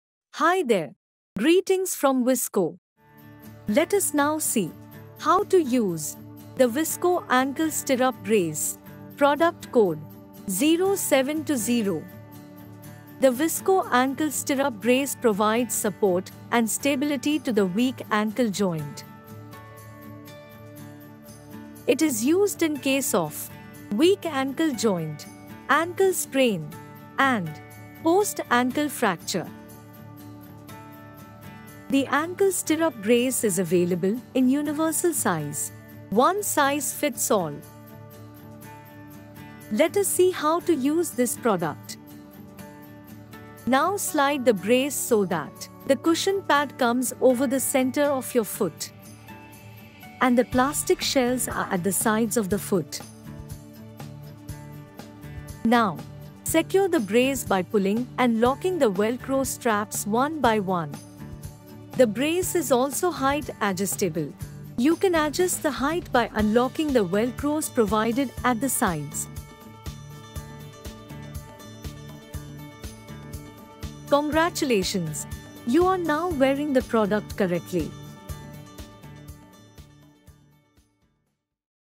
How to Wear & When to use Vissco Ankle Stirrup Brace
This is Video Guide on How to Wear & When to use Vissco Ankle Stirrup Brace (PC: 0720).

Ankle stir up brace can be used for:
• Ankle Injury
• Ankle Sprain
• Ankle Fracture
• Early Cast Removal
• Post-Operative Care

SCIENTIFIC DESIGN
• Scientifically designed to support the ankle and prevent injury.
• Improves ankle stability and provides support to prevent ankle injuries.
• The rigid padded contoured plastic shells give comfortable and optimum support to Ankle joint, Allows plantar flexion and dorsiflexion.
• Soft cushion inner linings protect against bony prominences and injured area other than providing comfort.
• Heel strap with Velcro help adjust height of the splint as per patient need and the product can be worn inside the shoe.